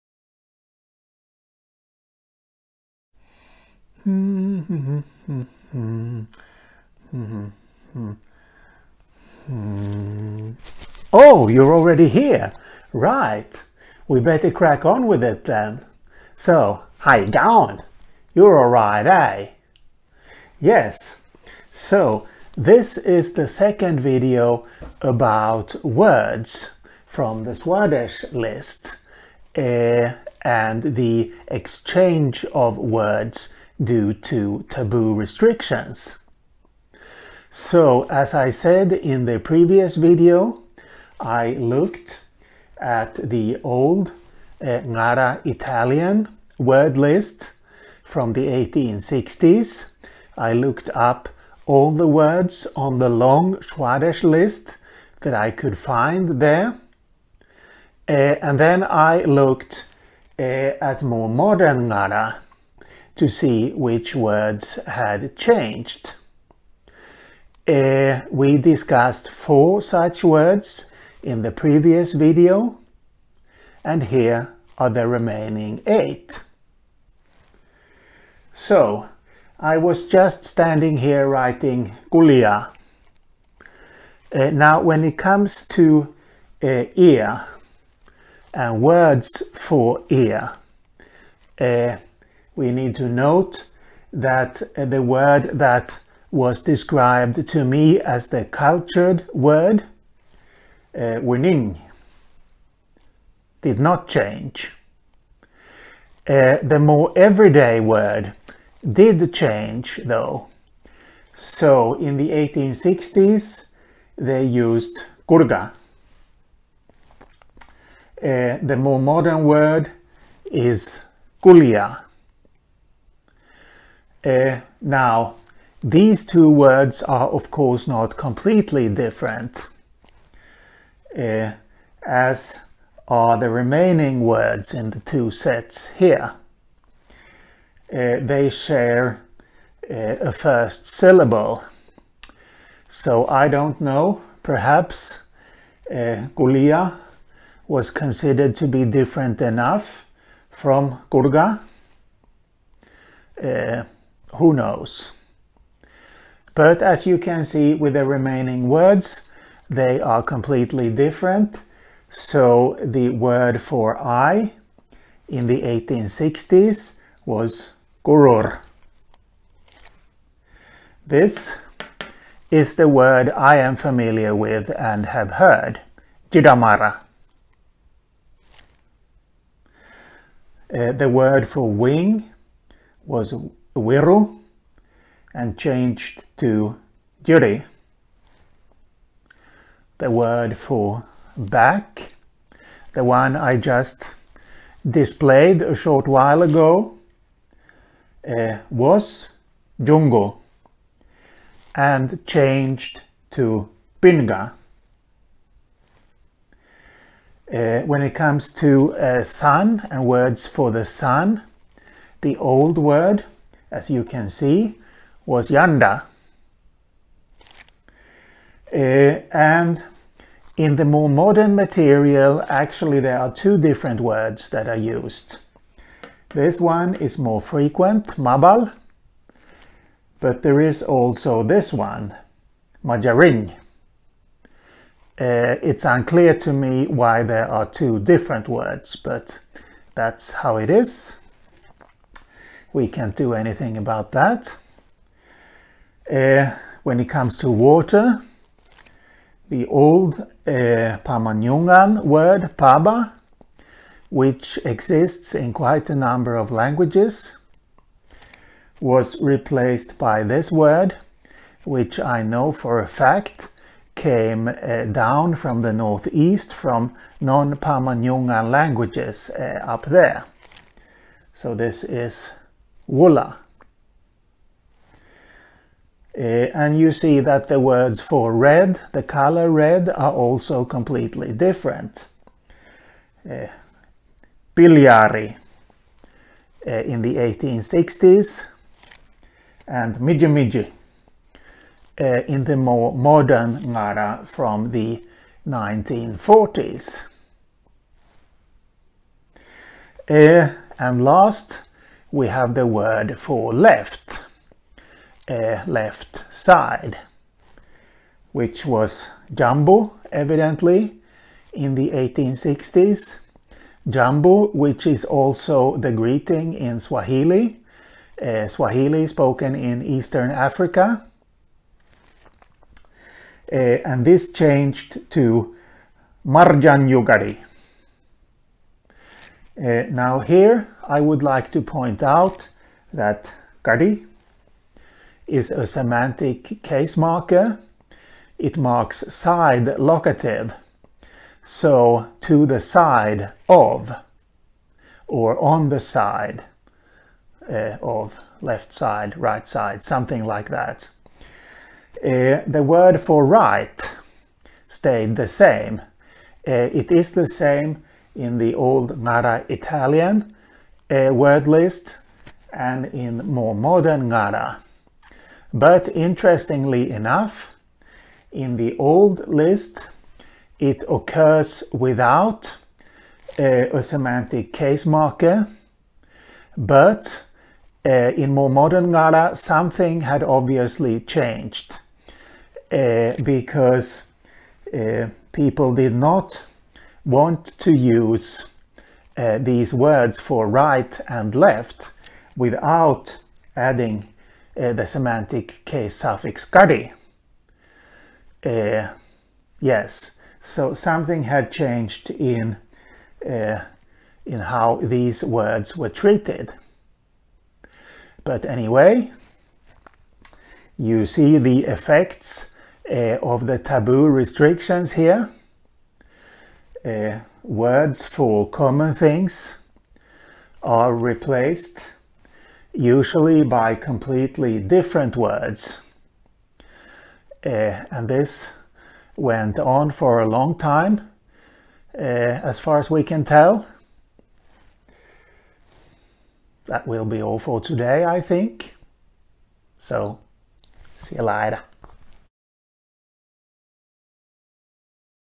Australian Words 5: Old & New Words 2 (Learning about Aboriginal languages)
This is the second video in which, as a way to illustrate the flux of the vocabularies of Australian Aboriginal languages, I look at the words of the long Swadesh list, and how the words on this list changed in 19th and 20th century Ngarla, a Pama-Nyungan language of the Pilbara region of northwestern Australia.